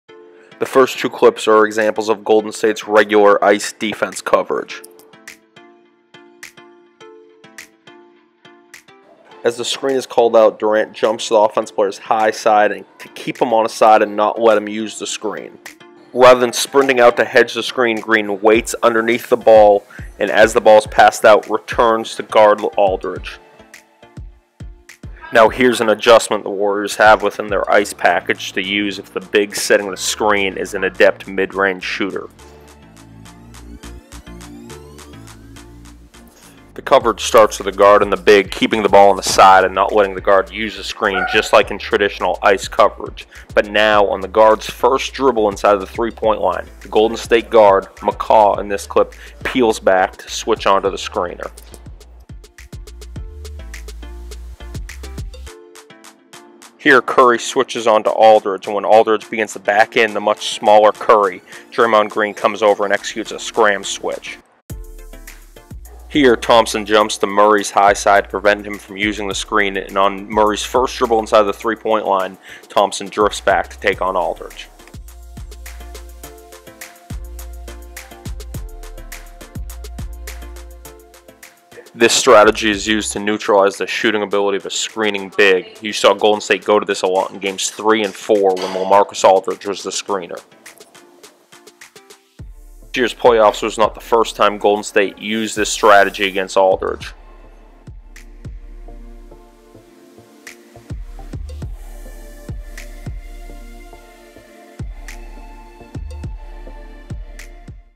ICE to Switch - Golden State Warriors Defense
Video compilation of Golden State's "Ice to Switch" concept